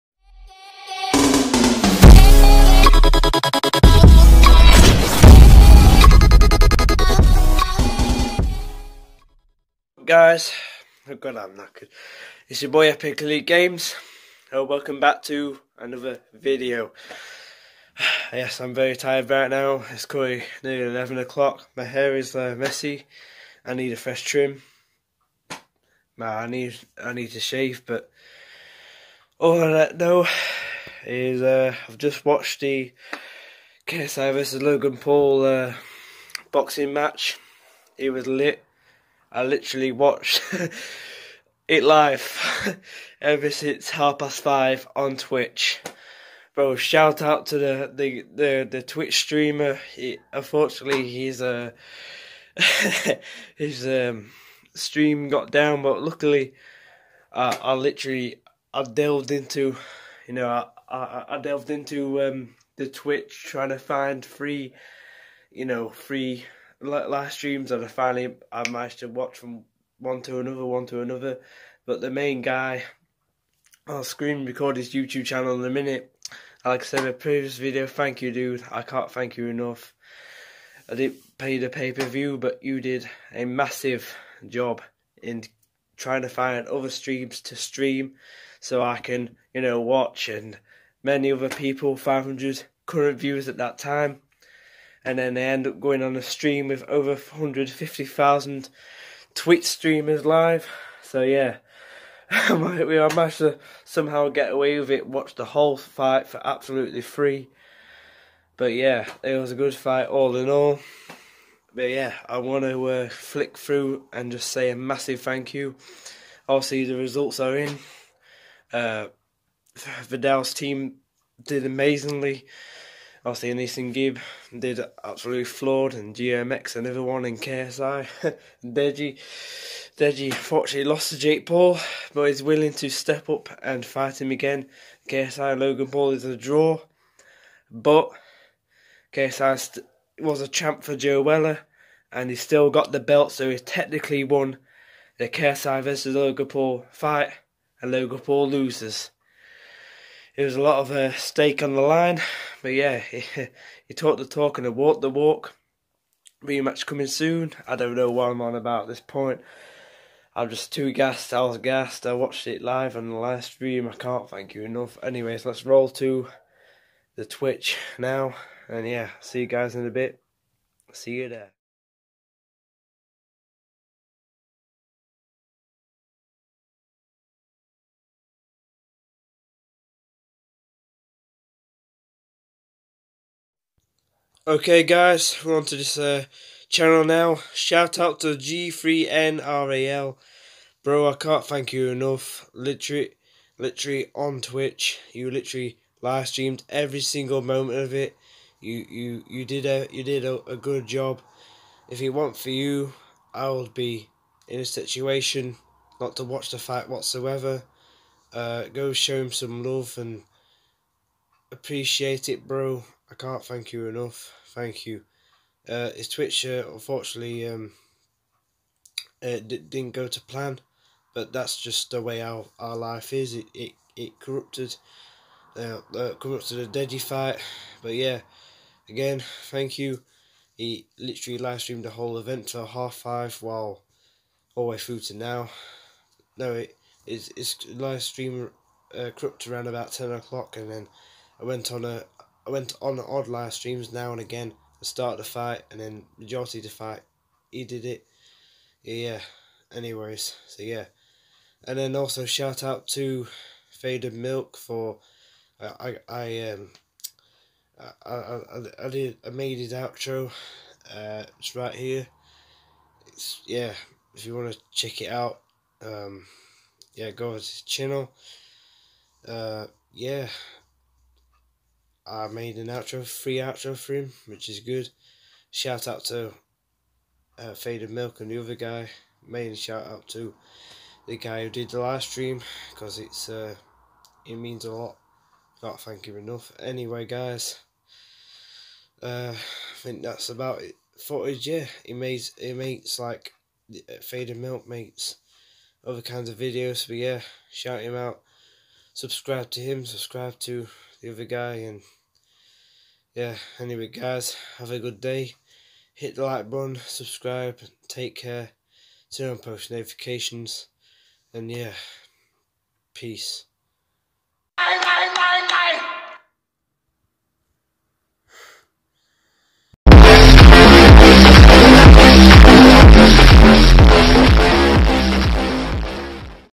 HOW I WATCHED THE KSI VS LOGAN PAUL BOXING MATCH FOR FREE **NOT CLICKBAIT**
Seriously its bot clickbait i pretty much watched it on twitch as other people was streaming it but I must say it was very difficult but it was worth it